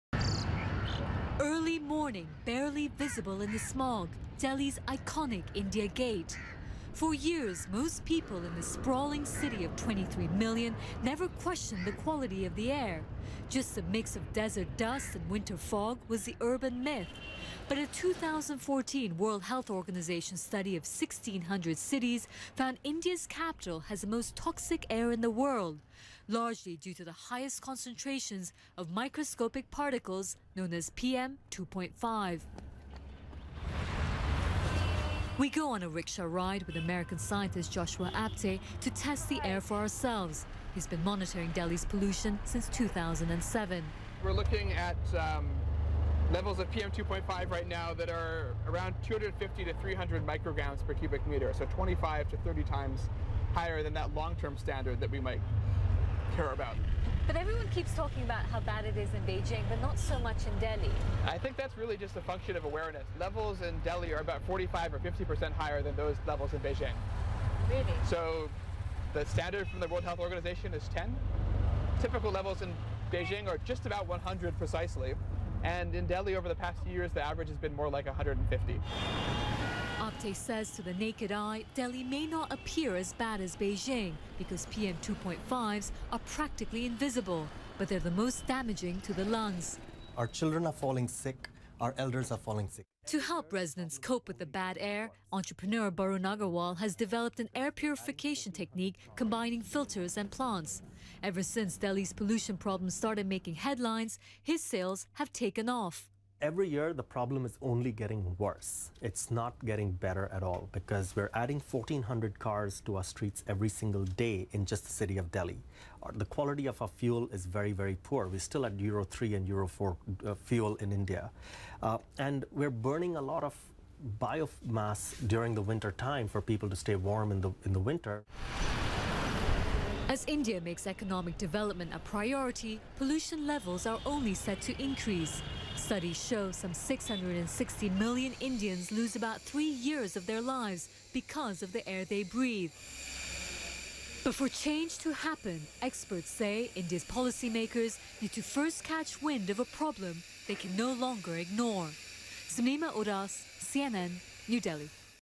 New Delhi has world's most toxic air
CNN's Sumnima Udas shows why the Indian capital, New Delhi, has the worst air quality in the world--- so bad, it shaves 3 years off your life.